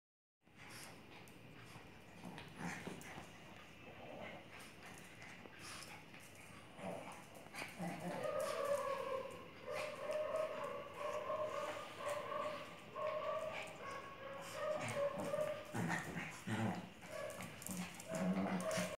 |||"Complitation of |||"best videos of @ LHASA APSO@ puppies and dogs in the world latest 2017-2018
|||"Complitation of |||"best videos of @ LHASA APSO@ puppies and dogs in the world.
Three lhasa apso family members are playing in this video.
Lhasa apso is a non-sporting dog breed originated in TIBET.
lhasa apso look like dainty toy dogs but are actualy strong and hardy watchdogs.
Lhasa apso is a breed of full of surprises.

 click on this link of lhasa apso dance video

This video is made by sehajpreet kaur from INDIA
Pets are like our family member.